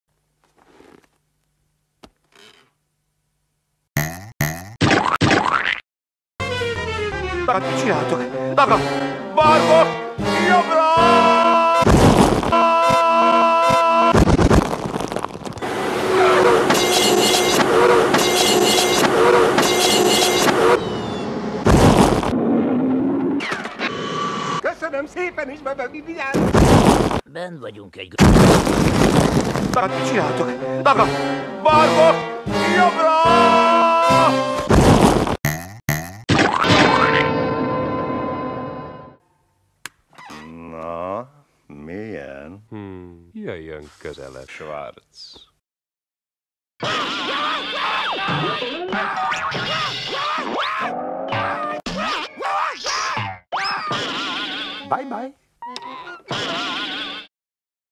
FunkyPoop: Von Schwartz bemutatja a Schwaroonitot!
Taiffel nagyon ideges attól, hogy valaki Scharoonitonak nevezi a saját cégét!